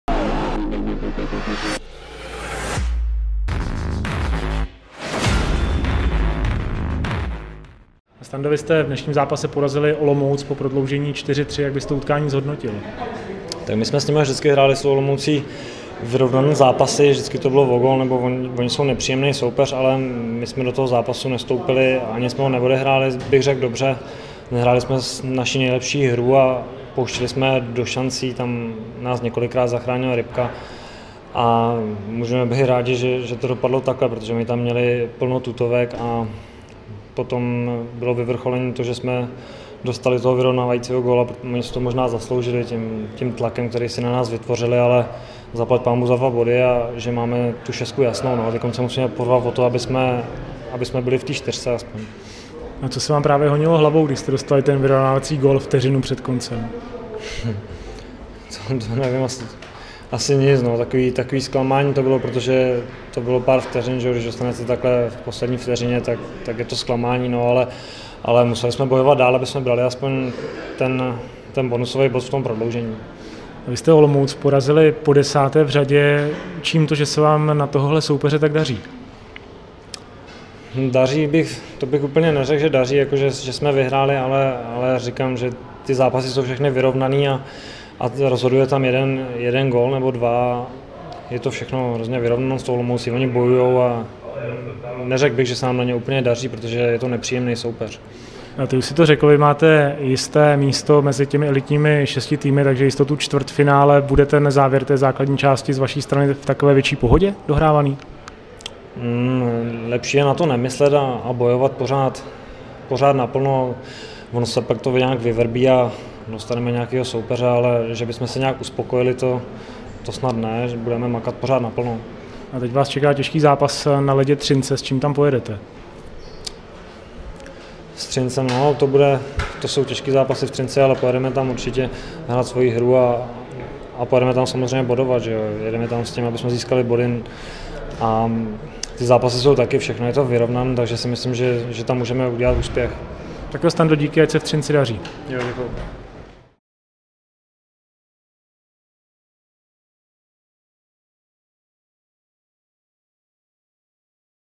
Stanislav Dietz po utkání Mountfield HK – HC Olomouc 4:3p
Někdy přijde zápas, který vám nevyjde úplně podle představ, a i přesto slavíte výhru. Takový byl podle Stanislava Dietze páteční duel 49. kola proti Olomouci. Východočeši drželi dlouho slibný náskok, ale v poslední vteřině o něj přišli. „Zápas jsme neodehráli dobře, nebyla to naše nejlepší hra,“ potvrzuje vytáhlý bek. Teď už ale vzhlíží k utkání proti Třinci a k udržení pozice mezi nejlepšími čtyřmi mančafty.